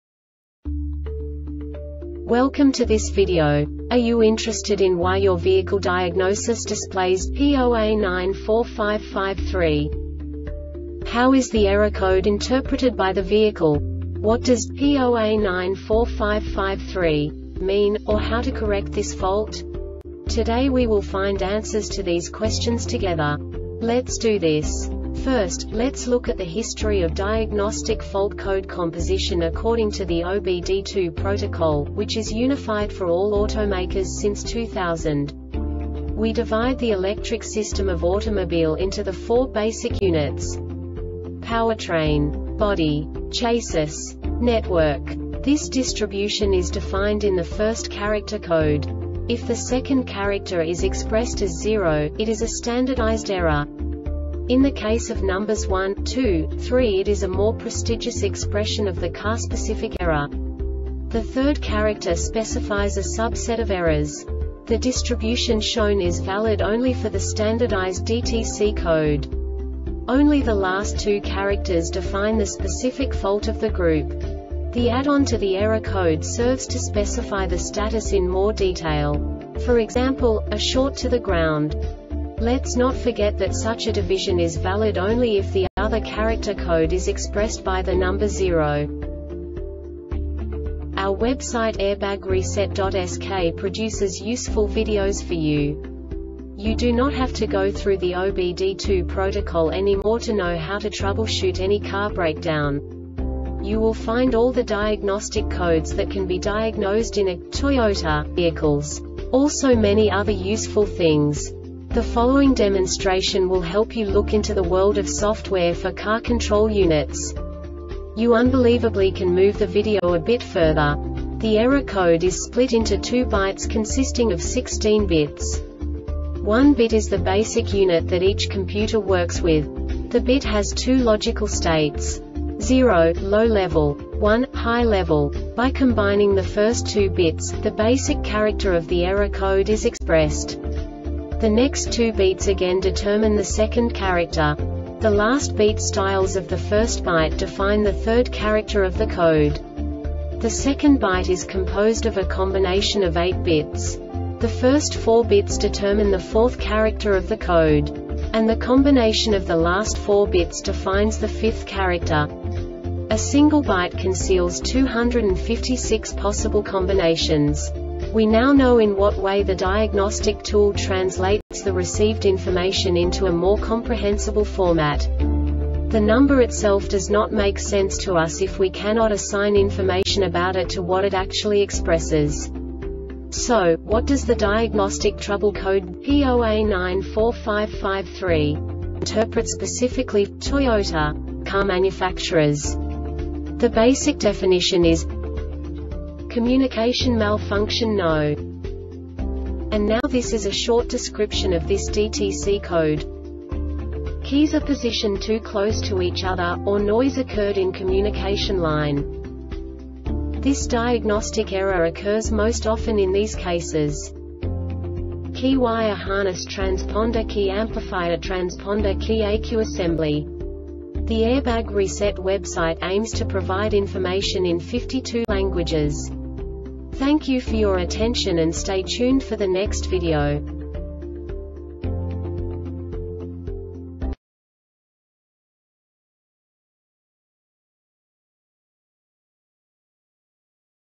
DTC Toyota P0A94-553 Short Explanation
Contents:
0:21 Basic DTC analysis according to OBD2 protocol standard.
1:48 Insight into programming
2:58 A diagnostically understandable description of the Diagnostic Trouble Code
3:05 Basic error definition

Make:
Toyota
Code:
P0A94 553
Definition:
Communication Malfunction No. 1
Description:
Keys are positioned too close to each other,or noise occurred in communication line.
Cause:
 Key Wire harness Transponder key amplifier Transponder key ECU assembly